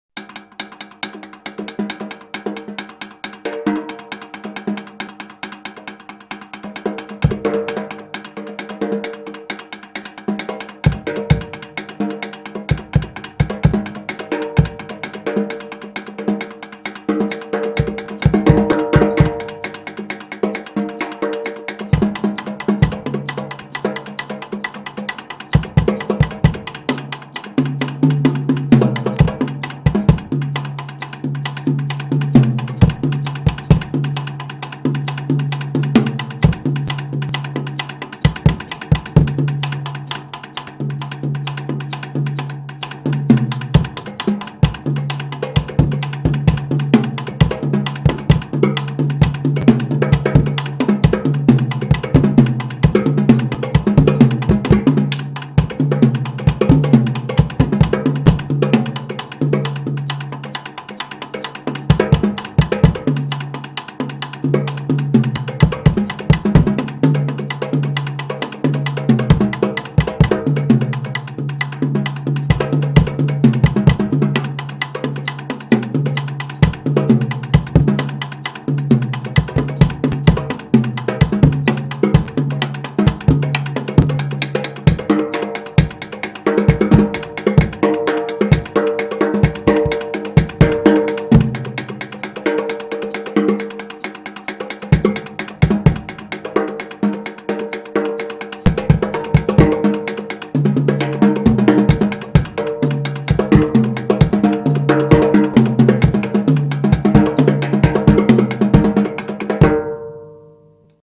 SnareSnap.mp3
Testing my bluetooth transmitter / receiver. Shure SM58 used as room mic into analogue mixer then bluetooth tx to rx on a pc line in.